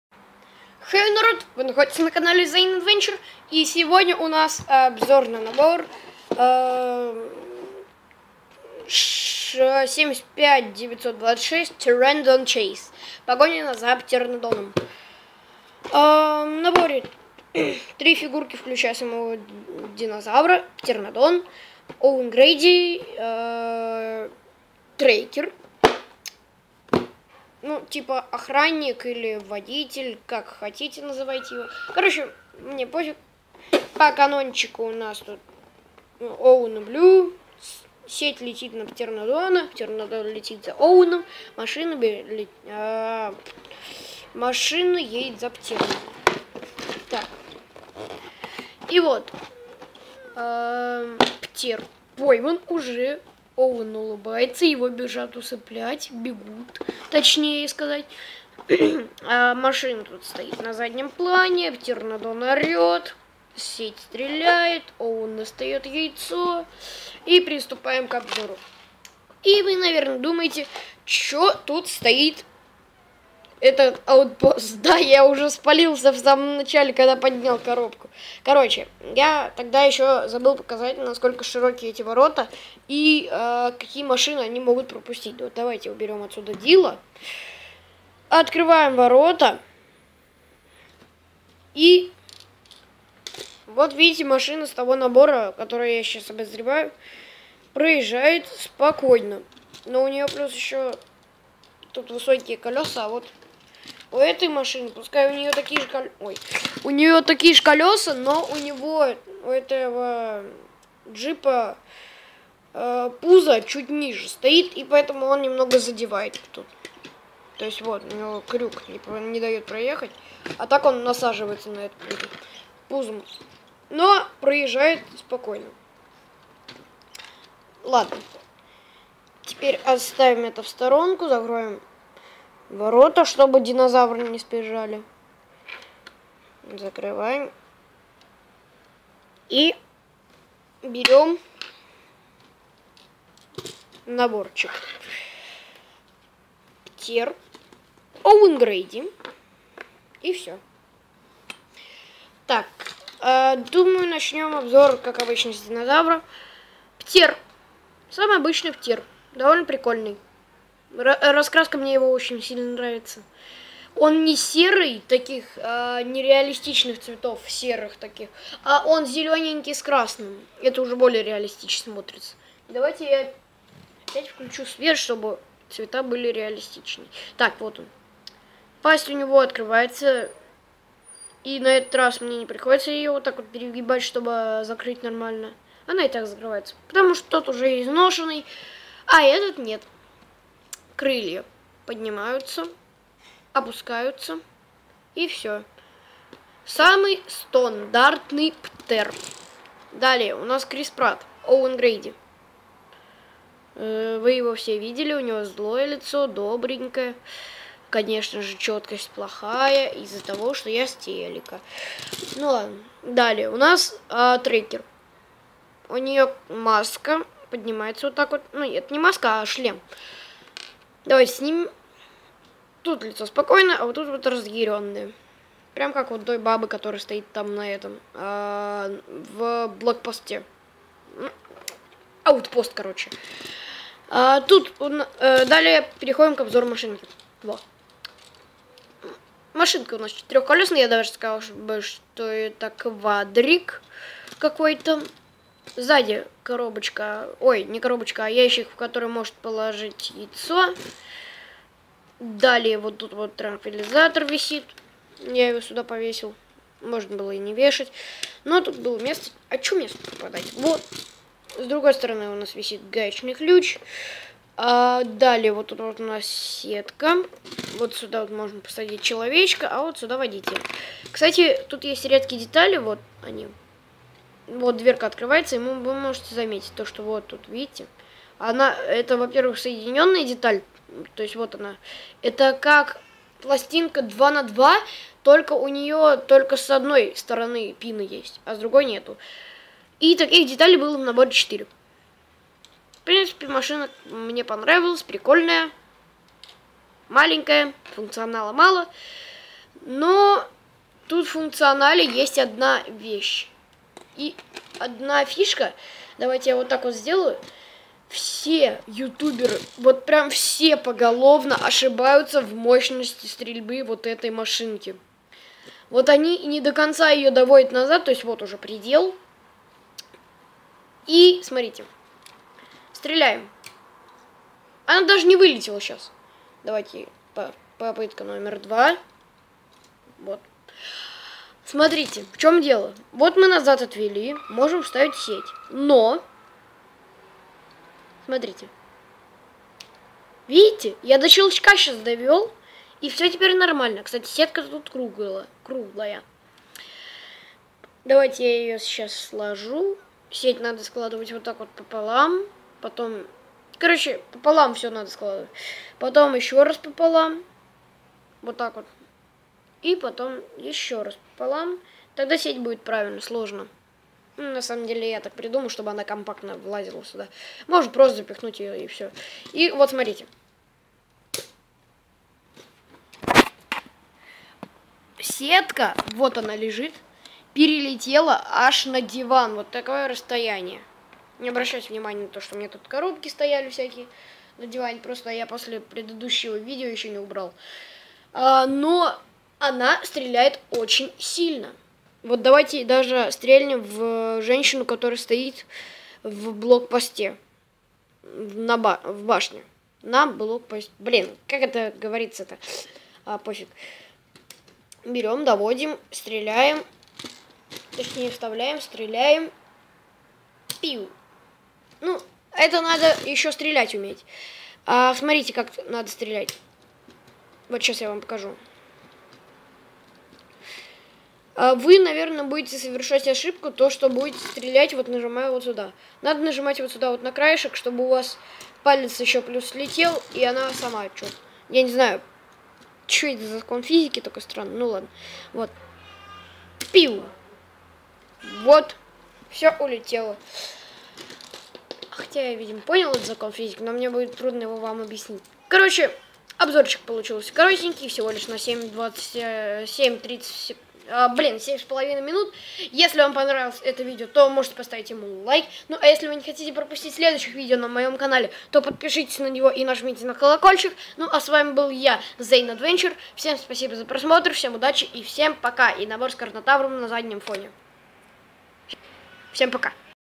Обзон на лего мир юрского периода 2 погоня за птеронадоном
Это очередной обзор на лего мир юрского периода и на этот раз набор погоня за птеронадоном